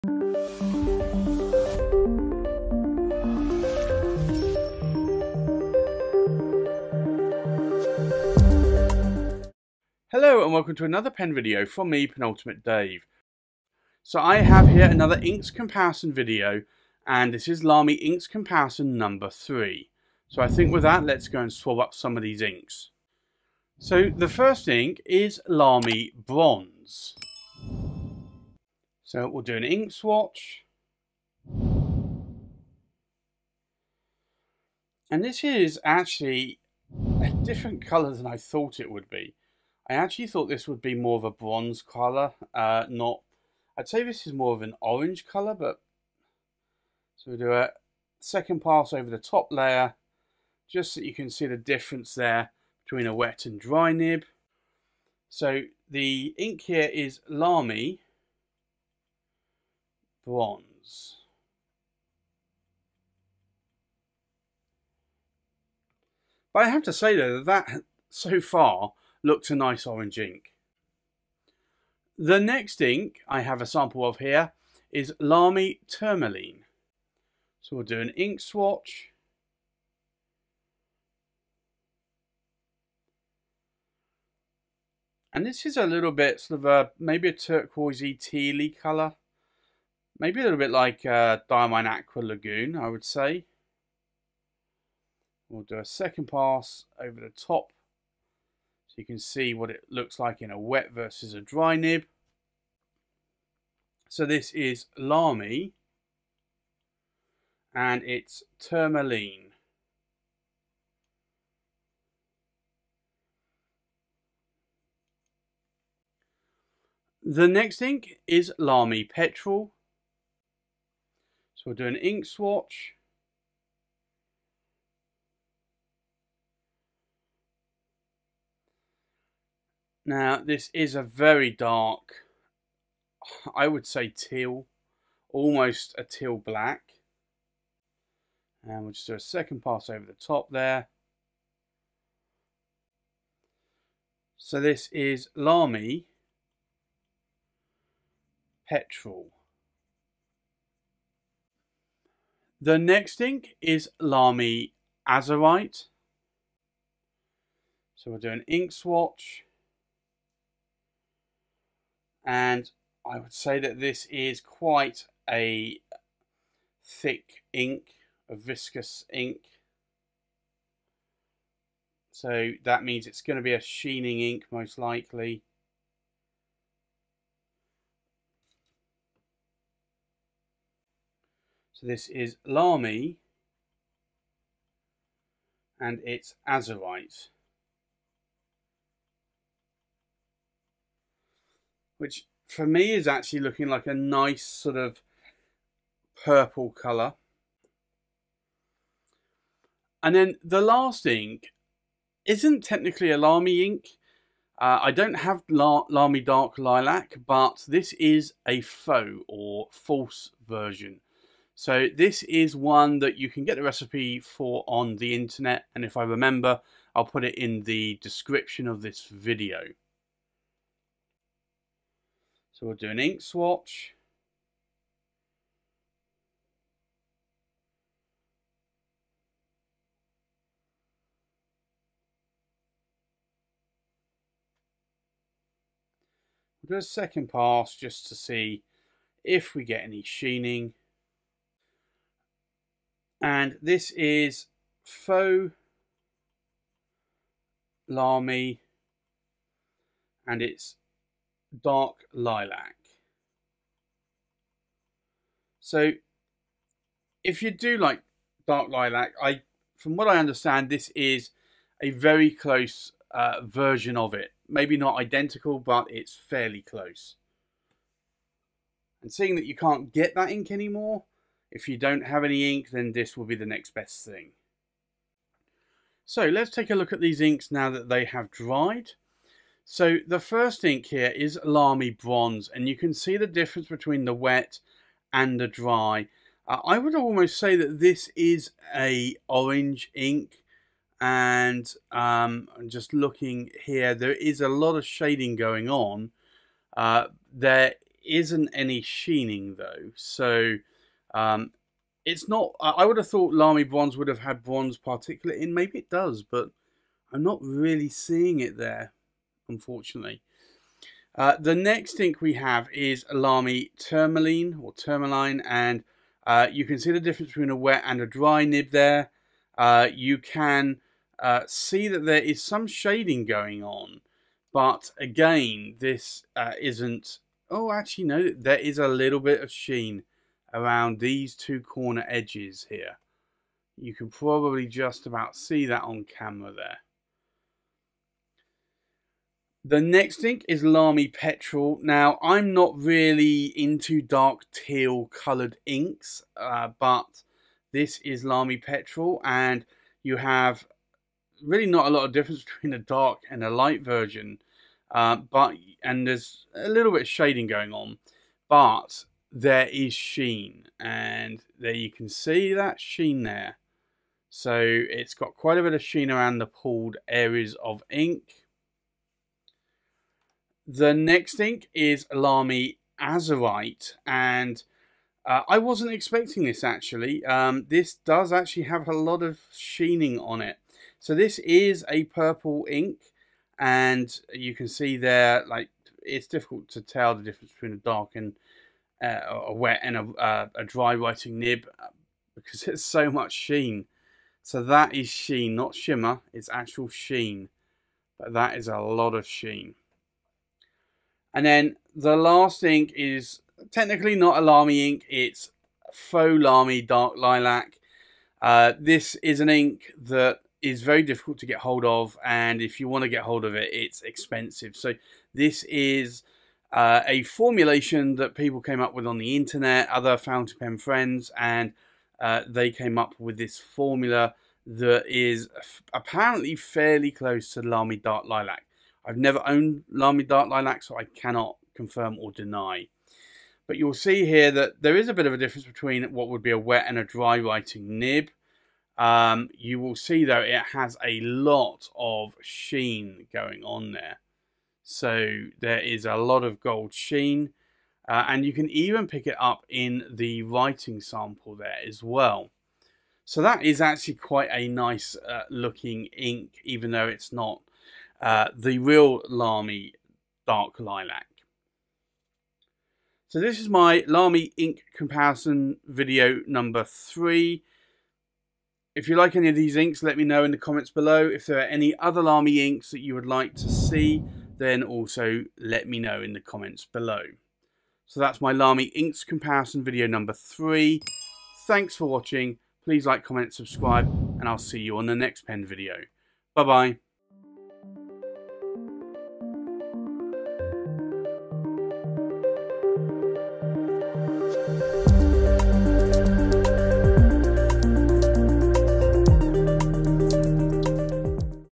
Lamy Inks Comparison #3
In this video I compare my Lamy Fountain Pen Inks that I have in my collection.

Inks in this comparison:-
- Lamy Bronze
- Lamy Turmaline
- Lamy Petrol
- Lamy Azurite
- Lamy (Faux) Dark Lilac

Lamy (Faux) Dark Lilac Recipe
The mix is Iroshizuku Yama-budo + Shin-Kai. The ratio is 3:1. If the mix is slightly too red, add a touch of Robert Oster Blue Denim.

The pen used in this ink comparison is the William Shakour Titan Dip Pen, it’s a pen made by a UK Pen Collector William Shakour.  I have a review of several of William's Titan pens including the full size and dip pens along with other accessories.